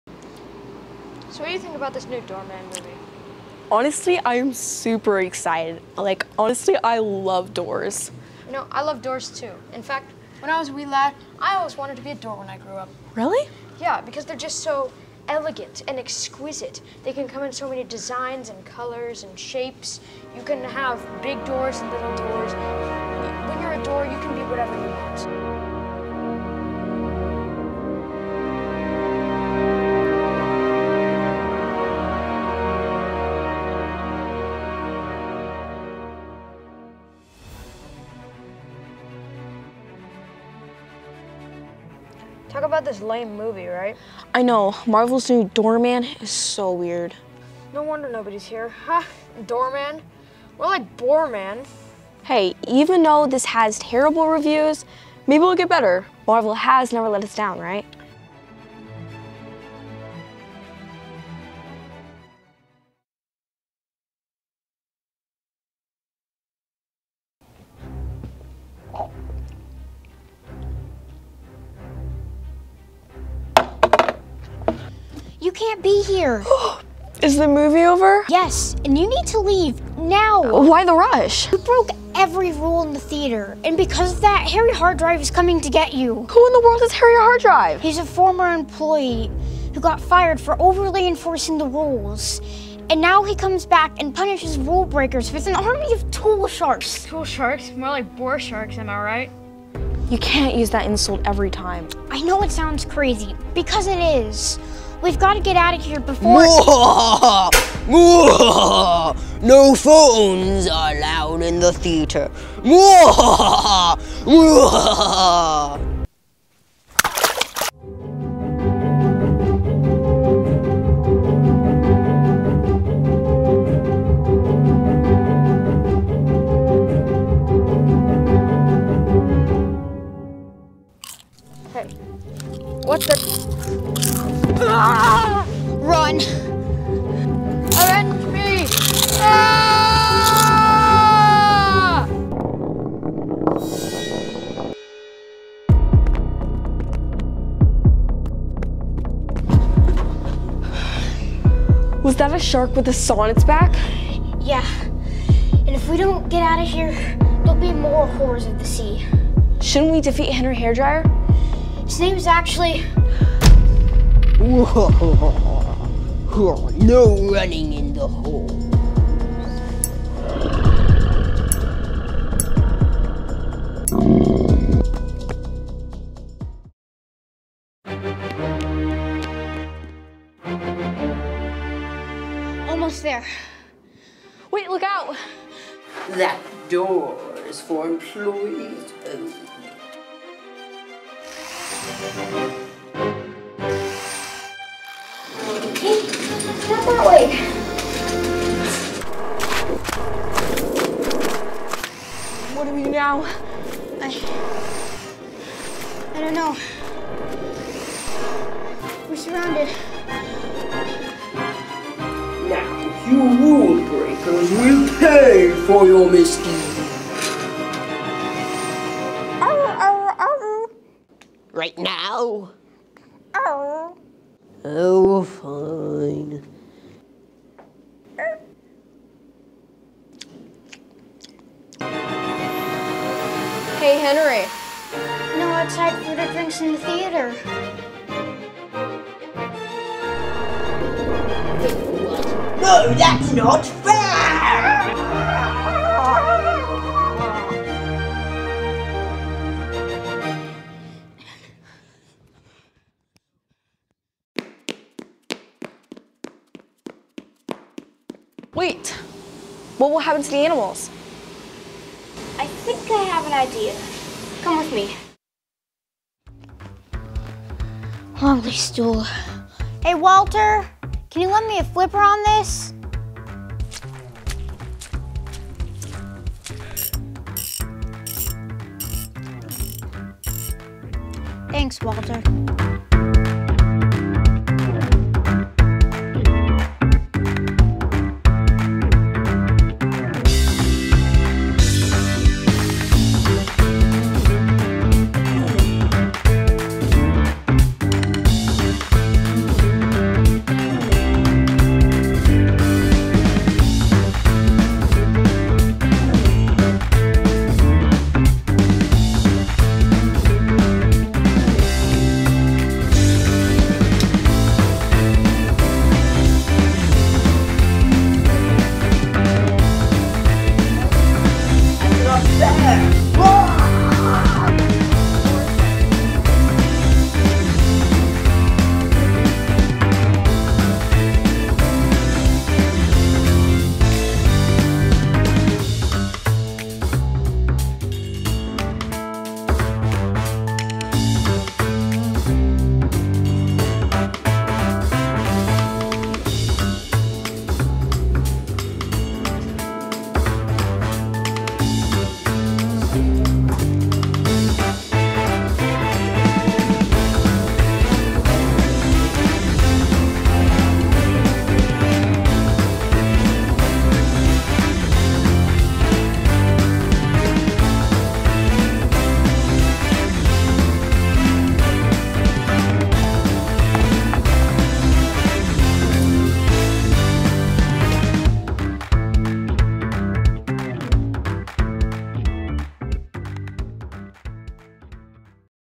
Harry Hardrive and the Horrors of the Haunted Hippodrome
Our original collaborative camp film made during the Young Filmmakers Camp at Alamo Drafthouse.
Theater goers, who break the theater rules, get reprimanded by an overly adamant former employee and his army of Tool Sharks.